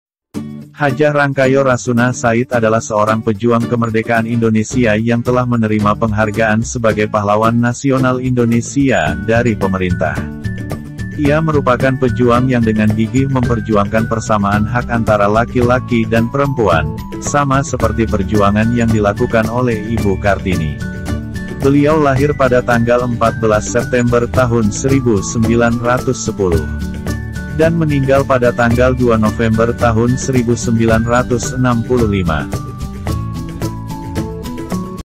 HR Rasuna Said
Hajjah Rangkayo Rasuna Said adalah seorang pejuang kemerdekaan Indonesia yang telah menerima penghargaan sebagai pahlawan nasional Indonesia dari pemerintah.